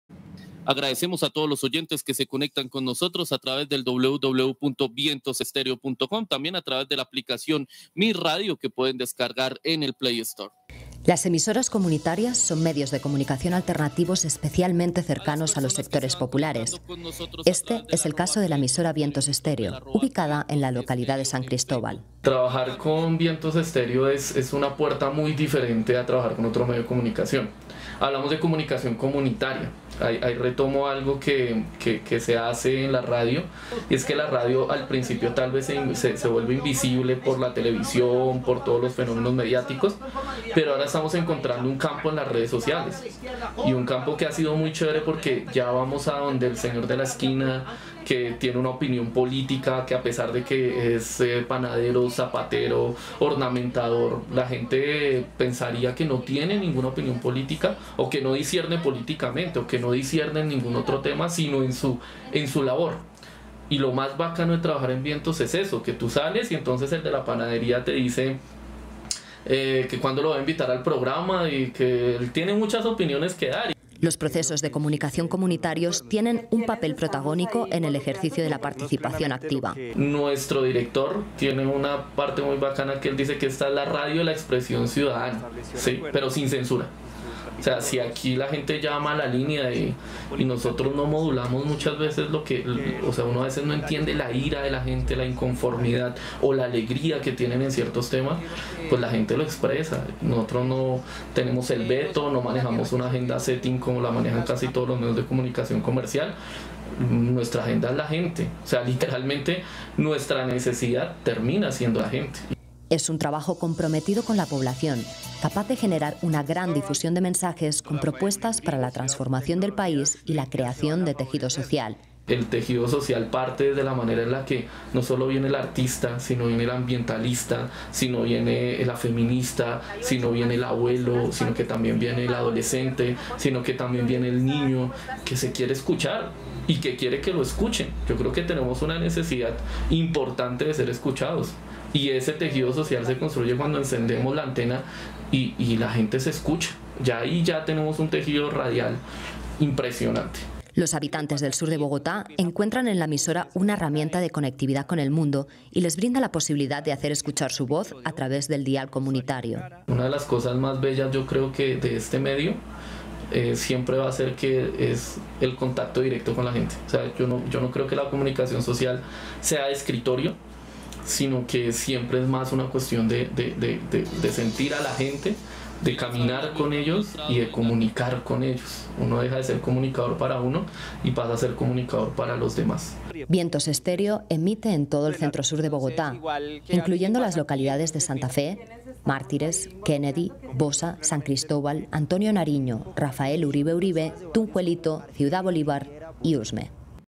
Vientos Stéreo, la radio comunitaria que construye tejido social en Bogotá | Capital En Línea
CapitalEnLínea | Las emisoras comunitarias son medios de comunicación alternativos especialmente cercanos a los sectores populares, este es el caso de la emisora 'Vientos Stéreo'. Esta ha sido su impacto en el sur de Bogotá